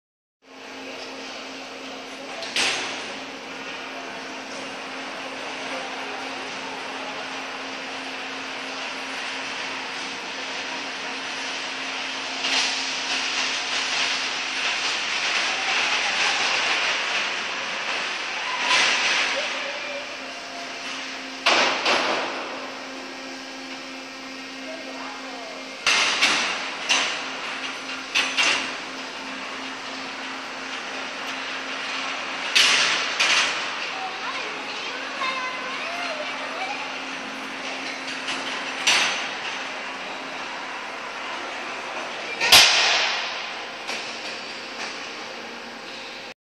big machine science museum cuenca
follow the ball as it rolls along to the bottom of an archimedes spiral. free on wednesday afternoons.